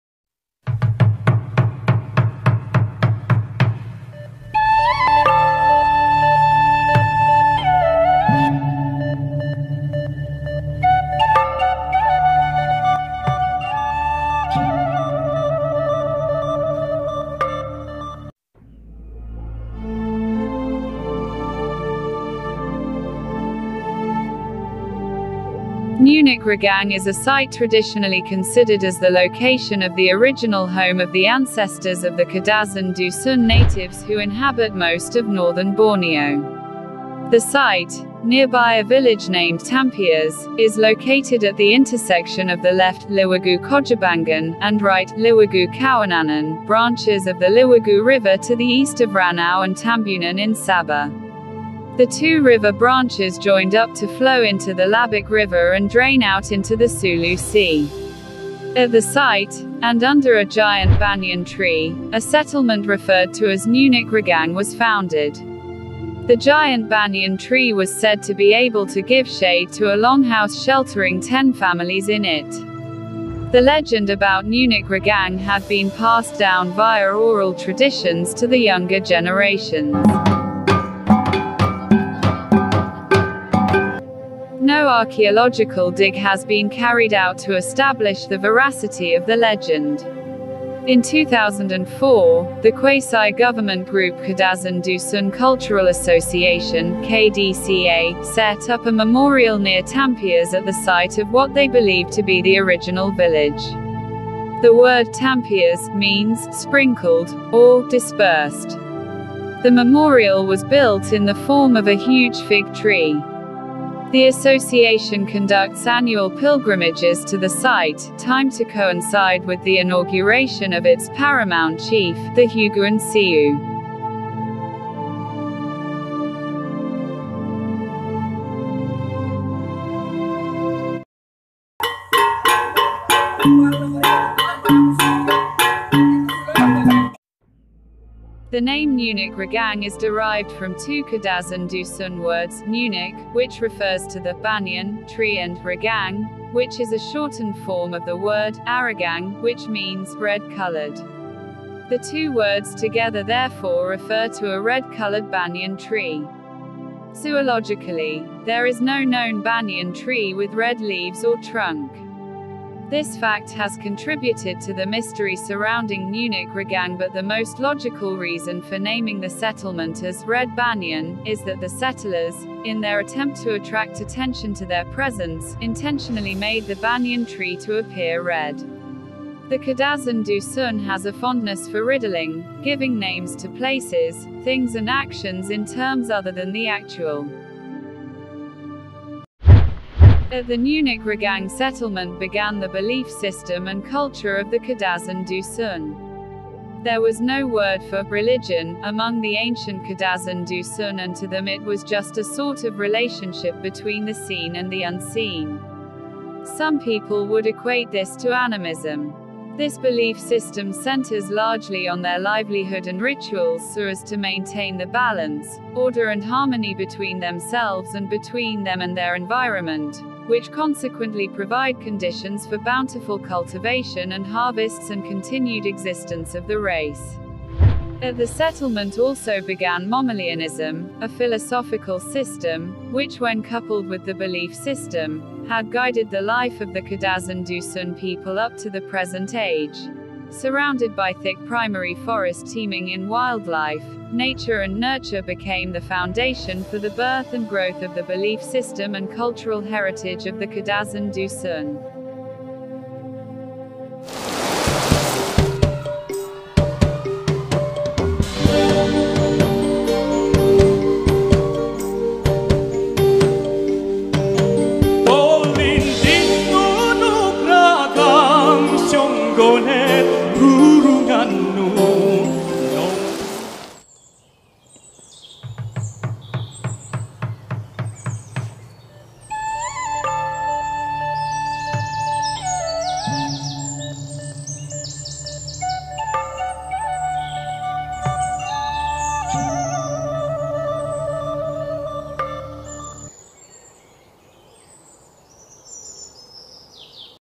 The Mythical Origin Of The Kadazan - Dusun Tribe ( Nunuk Ragang )
Nunuk Ragang is a site traditionally considered as the location of the original home of the ancestors of the Kadazan-Dusun natives who inhabit most of northern Borneo in Sabah , Malaysia.

The site, nearby a village named Tampias, is located at the intersection of the left (Liwagu Kogibangan) and right (Liwagu Kowananan) branches of the Liwagu River to the east of Ranau and Tambunan in Sabah.

The two river branches joined up to flow into the Labuk river and drain out into the Sulu Sea.

At the site, and under a giant banyan tree, a settlement referred to as Nunuk Ragang was founded.

The giant banyan tree was said to be able to give shade to a longhouse sheltering 10 families in it.

The legend about Nunuk Ragang had been passed down via oral traditions to the younger generations.

No archaeological dig has been carried out to establish the veracity of the legend.

✓ SANGAT DIALUKAN UNTUK THUMBS UP & KOMEN NYA DI BAHAGIAN BAWAH YA.